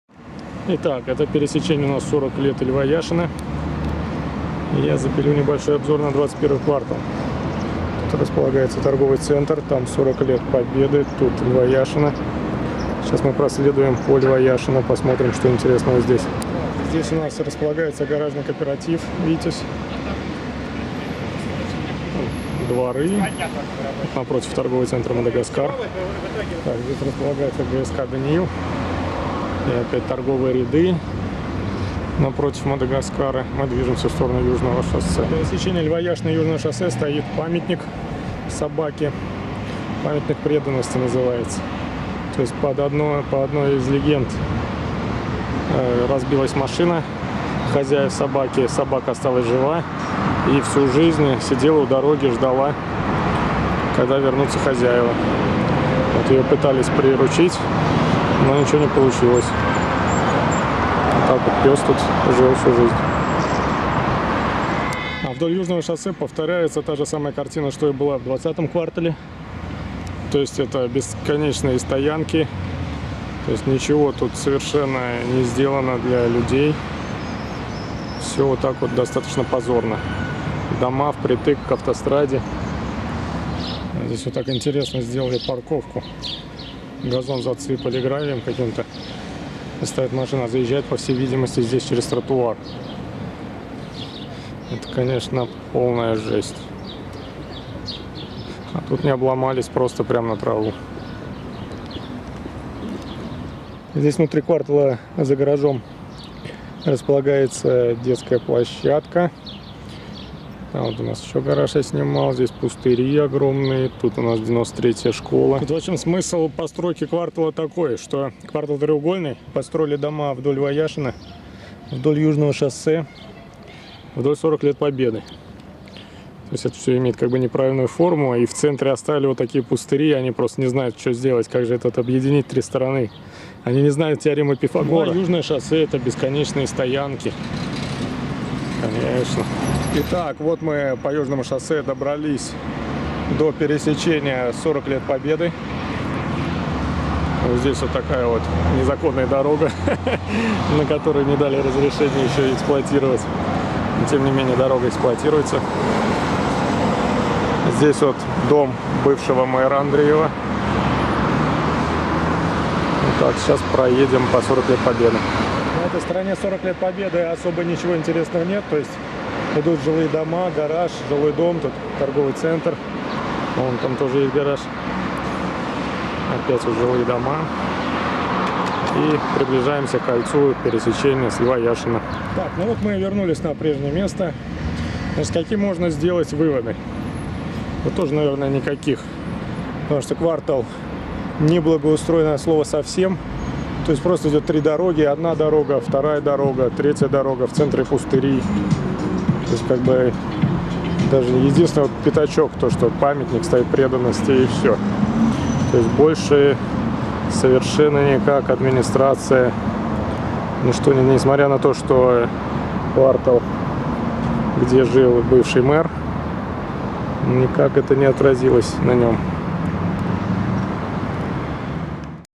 Обзор 21 квартала Тольятти
Пройдемся по улицам Льва Яшина, Южное шоссе, 40 лет Победы.